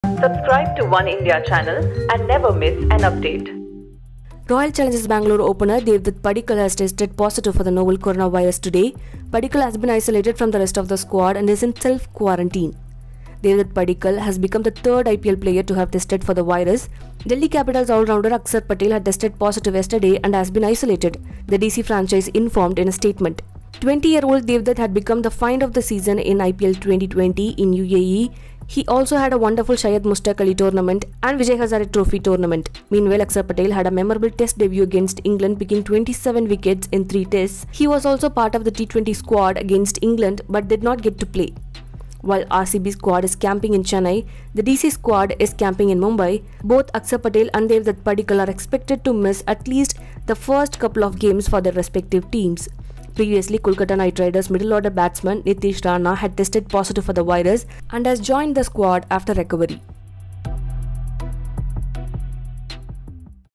Royal Challengers Bangalore & Delhi Capitals players test positive for Coronavirus | Oneindia News
Royal Challengers Bangalore opener Devdutt Padikkal has tested positive for the novel coronavirus today. Delhi Capitals allrounder Axar Patel had tested positive yesterday and has been isolated, the DC franchise informed in a statement.